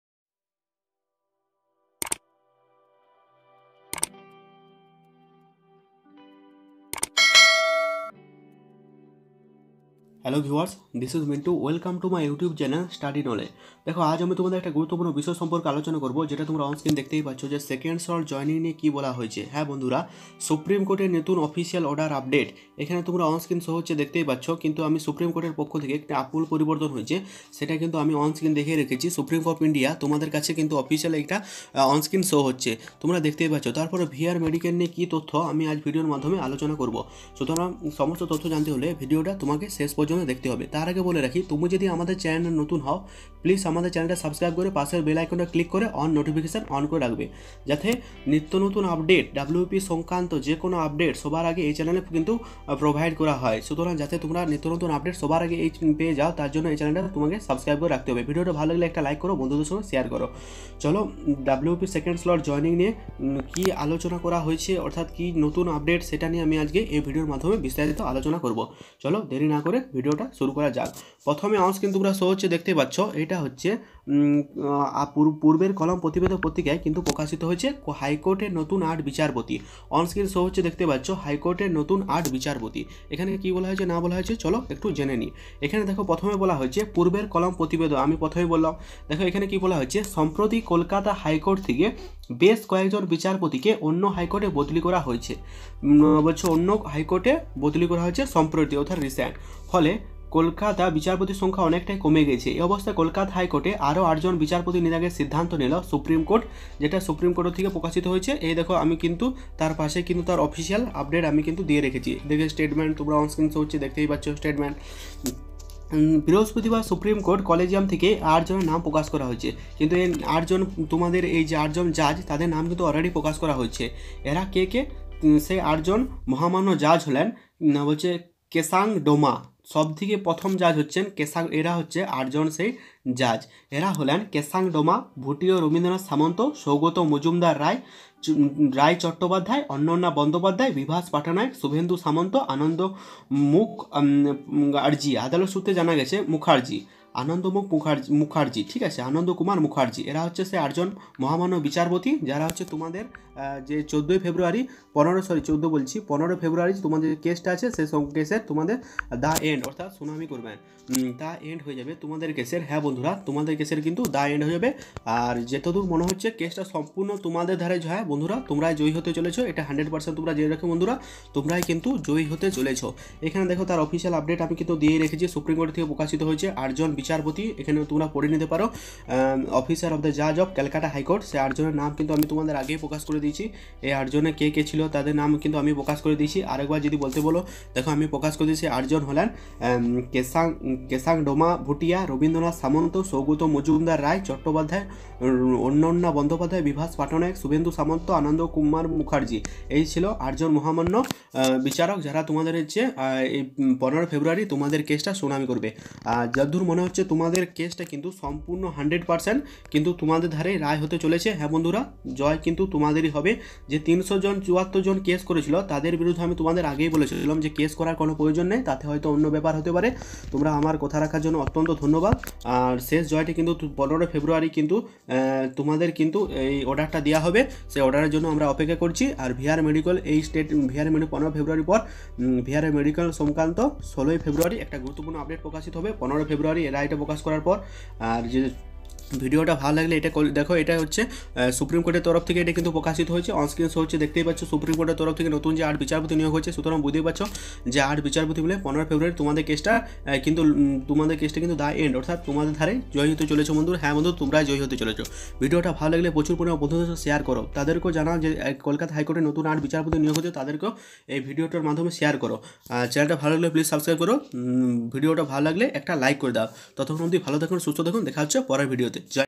The new 2nd slot joining update of WBP/15 ই feb🔥🔥🔥🔥
IN THIS VIDEO THE 2ND SLOT JOINING UPDATE OF WBP CONSTABLE UPDATE PUBLISHED.